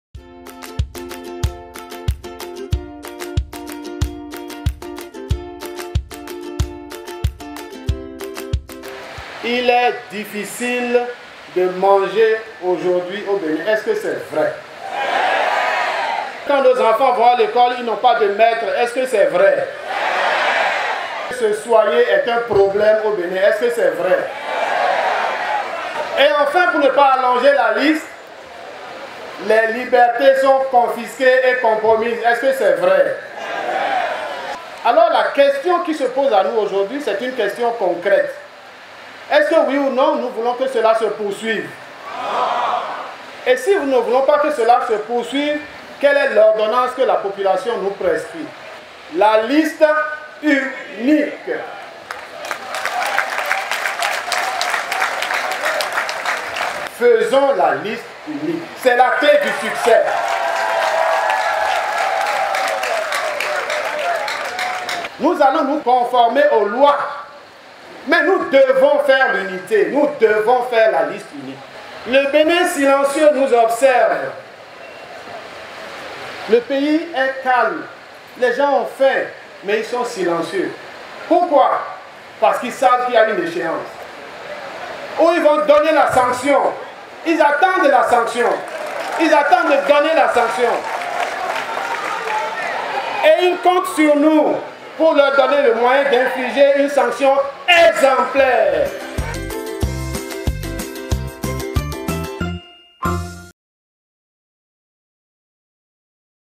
Congrès de l'USL : Eric Houndété entre réquisitoire contre le régime Talon et appel à l’opposition
Le 15 décembre 2018, le parti Union sociale libérale (USL) a tenu à Cotonou son congrès extraordinaire de mise en conformité aux dispositions de la nouvelle charte des partis politiques. Au cours de ce congrès, le vice-président du Parlement béninois, Eric Houndété a dressé un sévère réquisitoire contre la gouvernance Talon. C’était l’occasion pour lui d’appeler, dans une approche de pédagogie politique, les populations à soutenir la liste unique de l’opposition au pouvoir Talon lors des élections législatives de 2019.